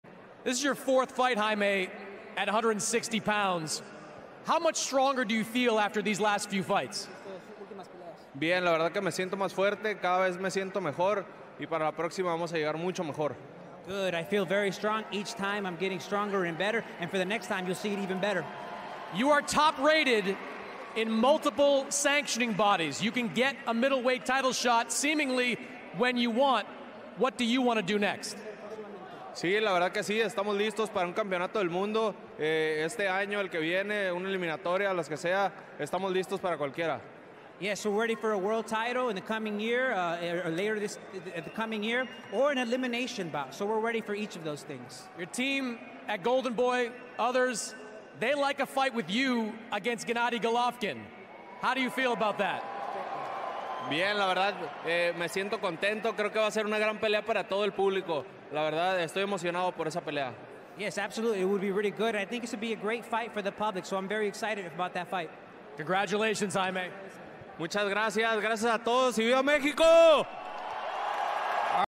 GGG vs. Munguía Next?
November 16th, 2021 -- Jaime Munguía discusses his world title aspirations in his post-fight interview against Gabe Rosado on November 13, 2021 in Anaheim, California.